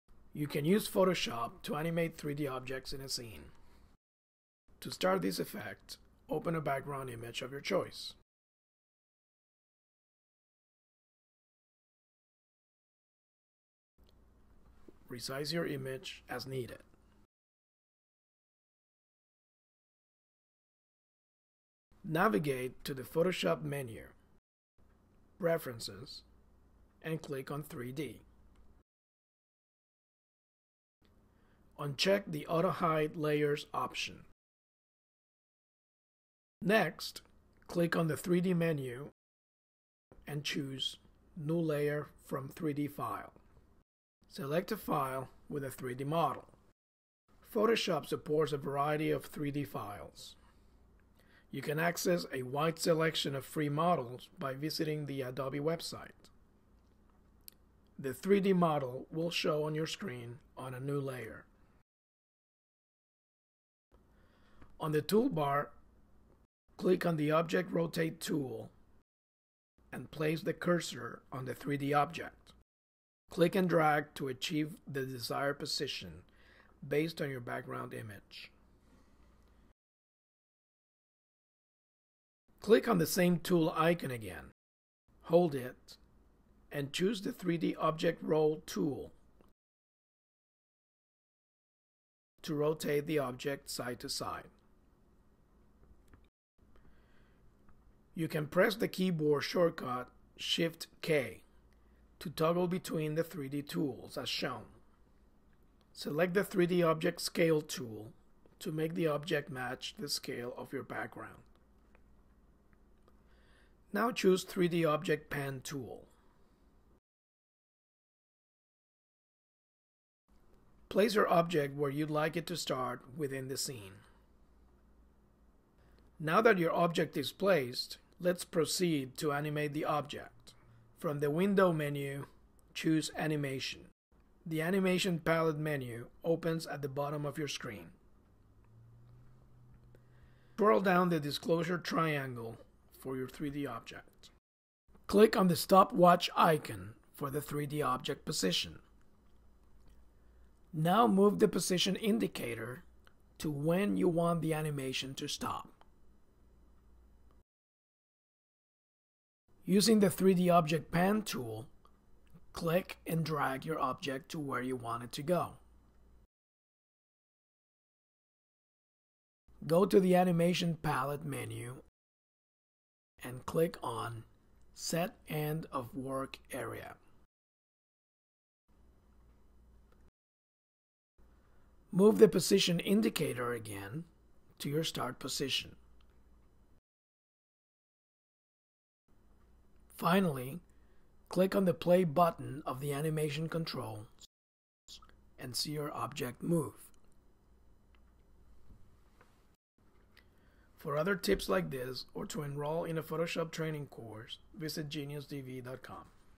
3D Animation in Photoshop
Import and animate 3D objects using Photoshop CS5.